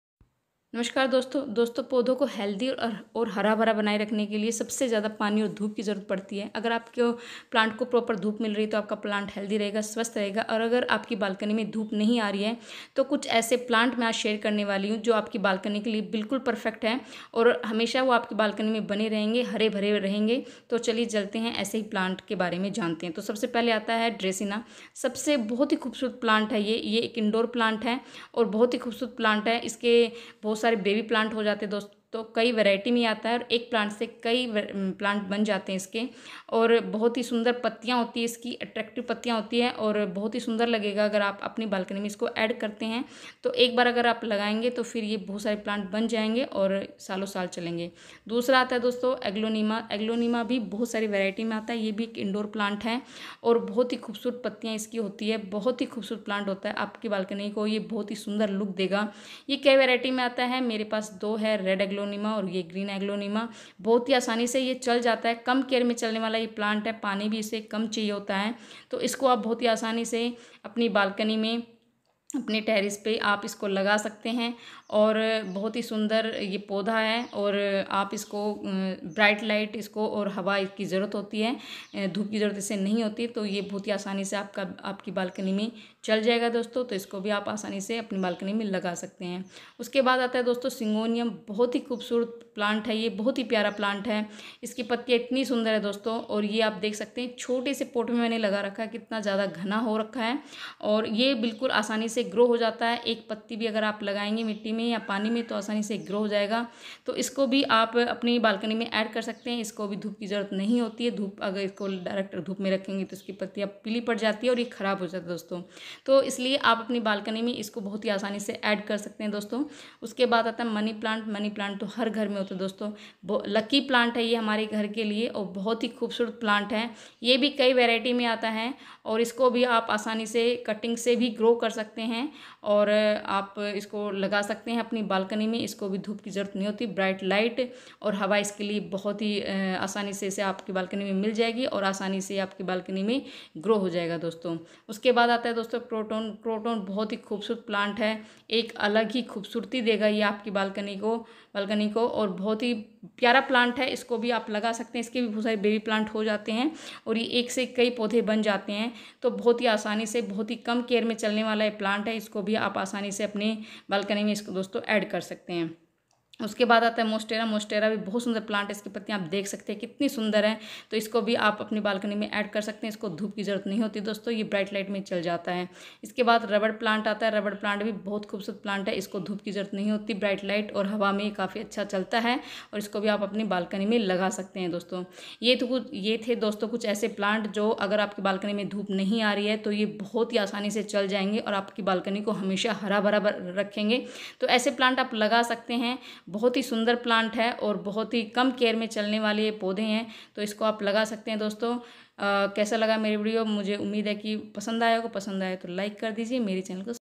आपकी बालकनी में नहीं आती धूप? तो लगाइए 6 पौधे साल भर बालकनी रहेगी हरी भरीplant without sunlight
Best indoor plants for Balcony
gardening with Santosh 🌴🌳